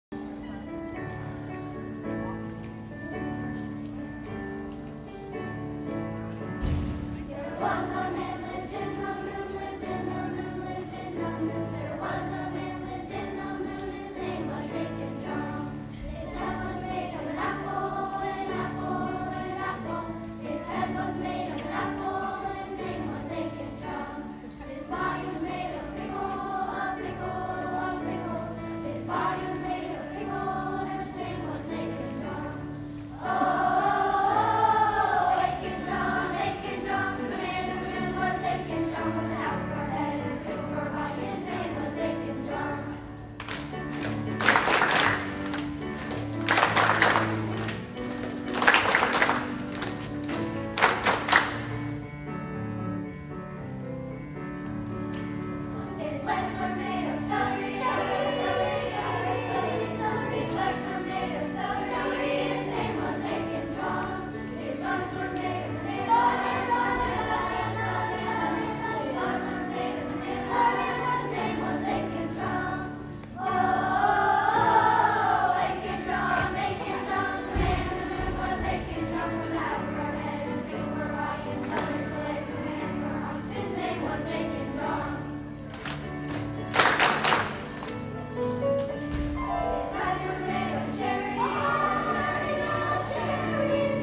Linden Elementary School Choir
Under the leadership of Ms. Young, this is the Linden Elementary Choir Hamilton, Ohio.